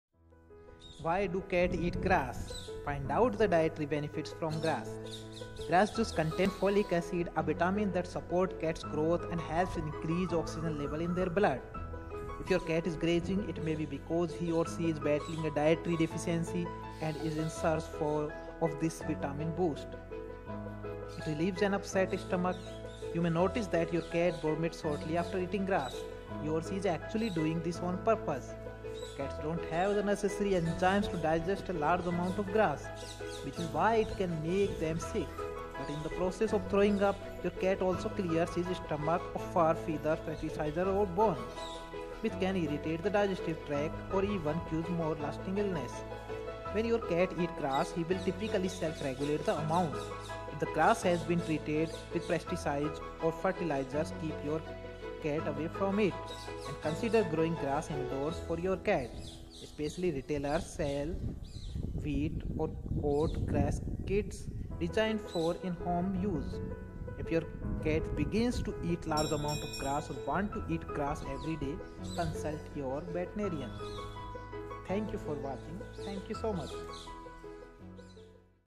Why do Cats Eat Grass?,
Eating Grass in Cats - Definition, Cause, Solution, Prevention, Cost
Why Do Cats Eat Grass? - Cat Health